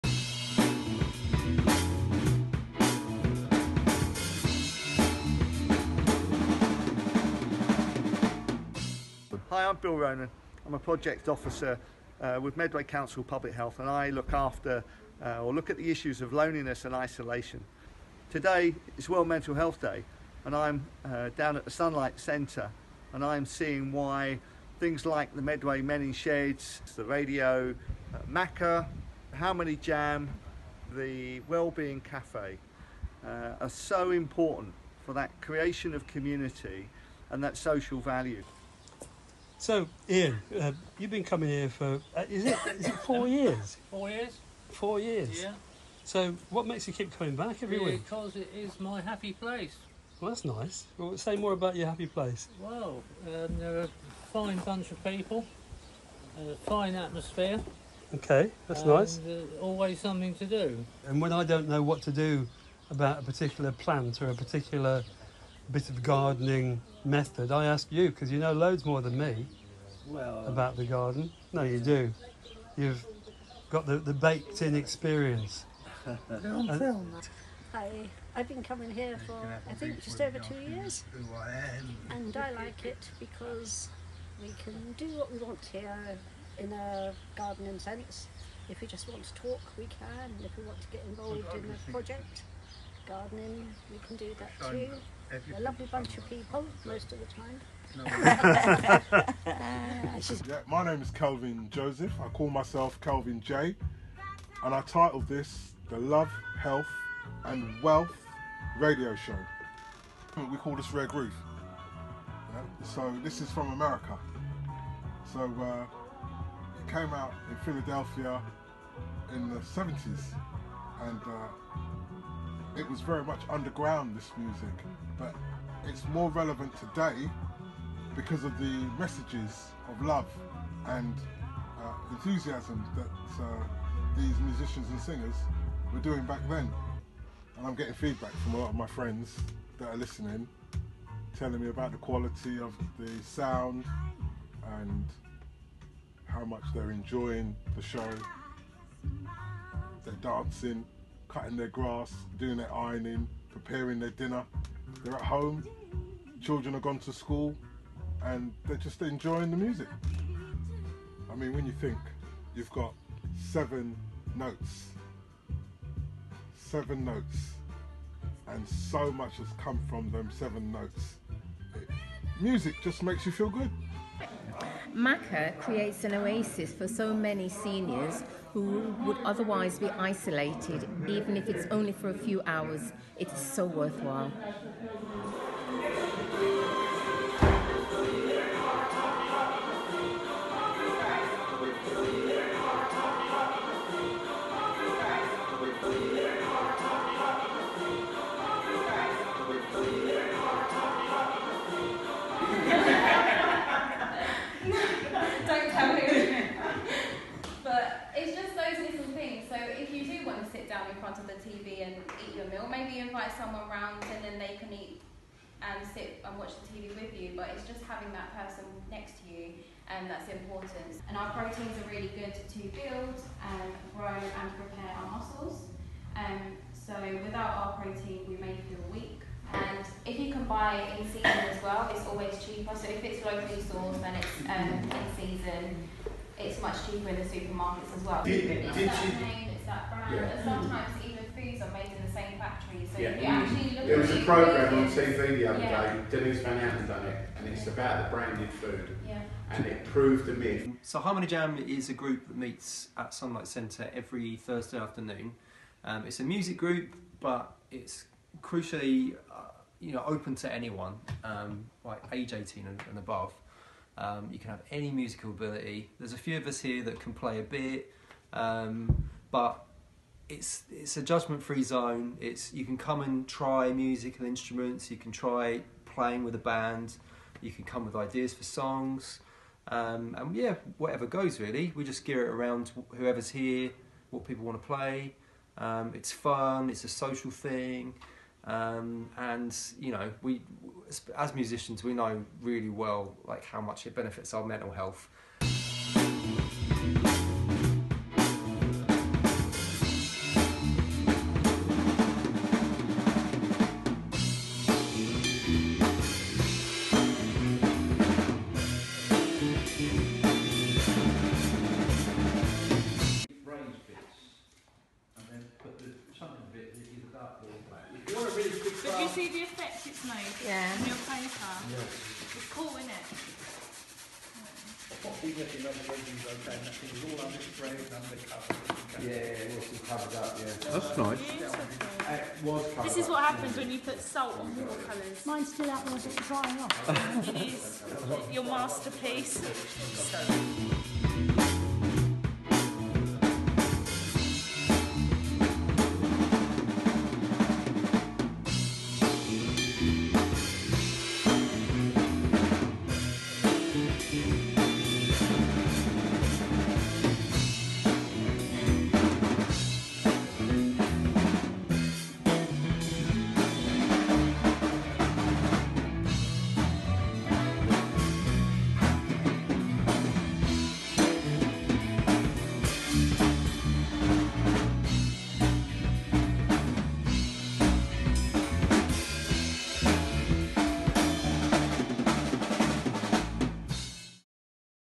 Sunlight World Mental Health Day
5 community groups and A better Medway celebrated World Mental Health Day at the Sunlight Centre in Gillingham, Kent.
The event gathered groups together with music, grow to eat gardening with butternut squash soup, home made fresh food in the hall, a talk on healthy food from nutrition experts and an art workshop revolving around 5 steps to wellbeing.